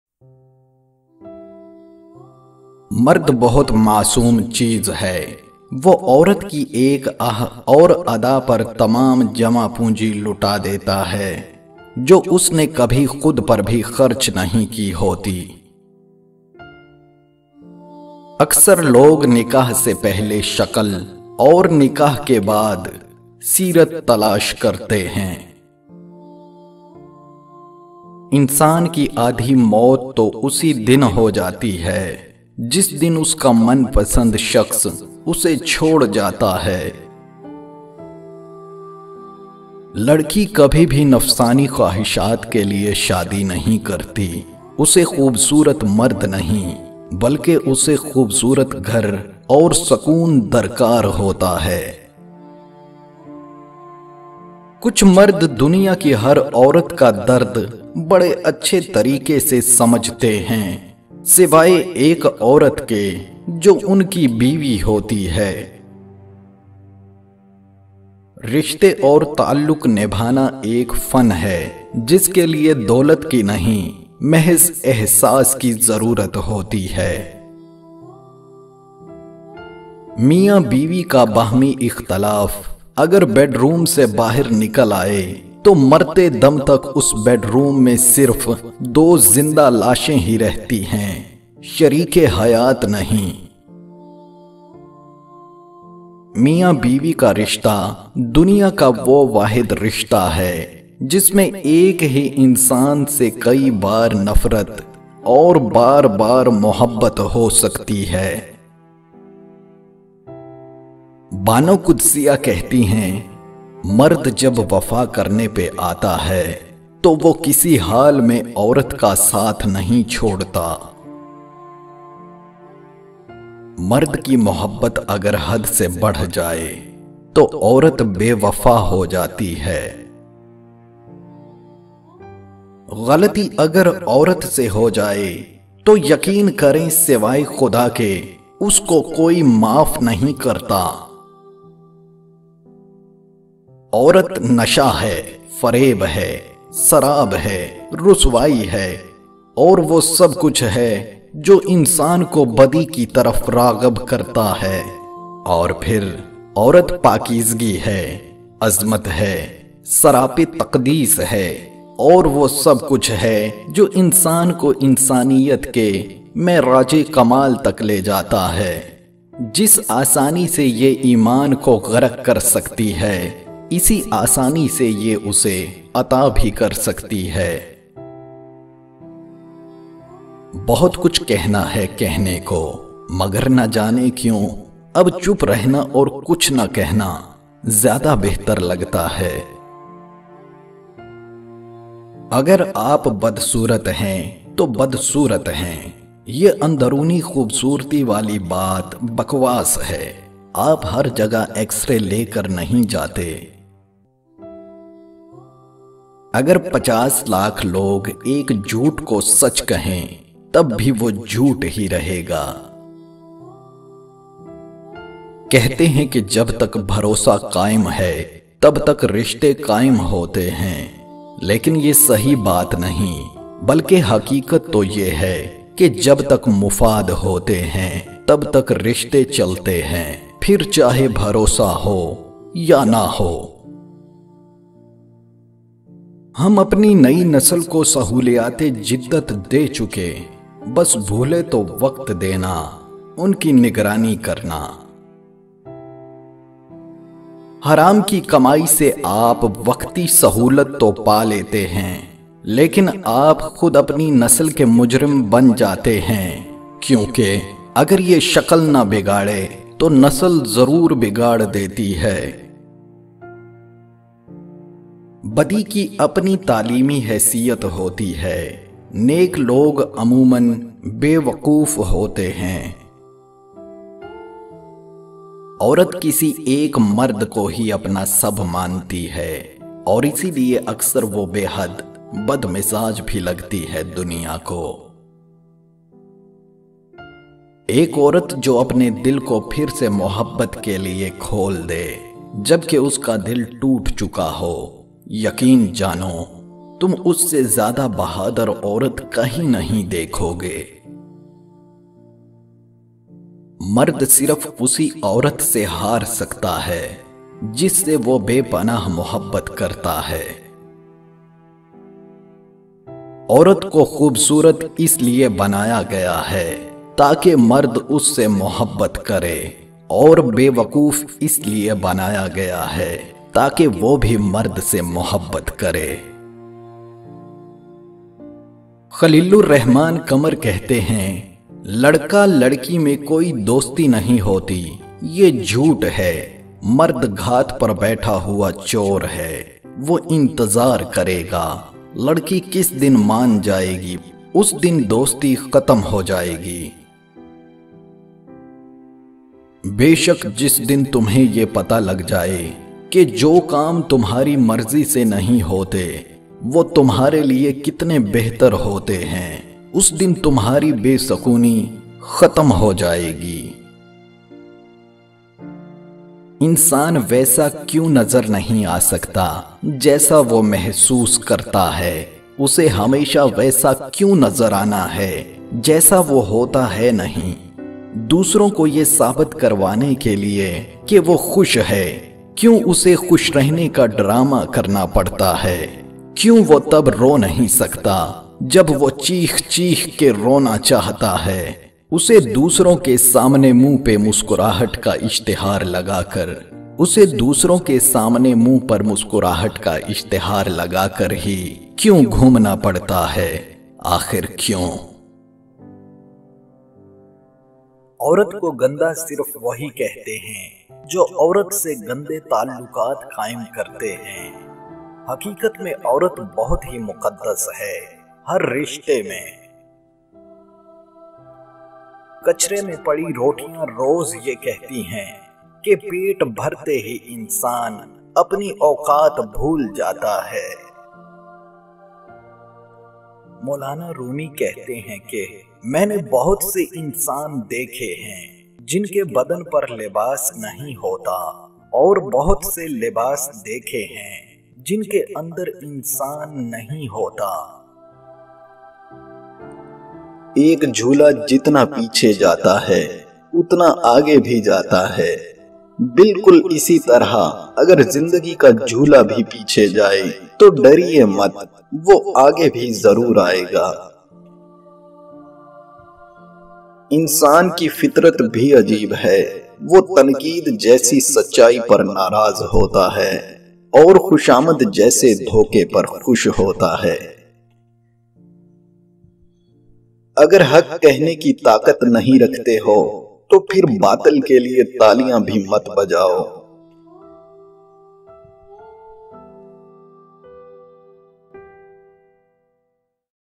Best Inspirational Hindi Quotes | Most Famous Quotes | Quotes About Love | Motivational Quotations
Piary Dosto,
Aj ap Most Famous Quotes, Quotes About Love jo k Best Inspirational Hindi Quotes hain, Sun k bohat enjoy karain gy. These are also Motivational Quotations that will encourage you in every matter of Life.
Note: This Video is Copyrighted & Property of " Rj Javaid Shahzad " YouTube Channel. So, Nobody Can Use This Video.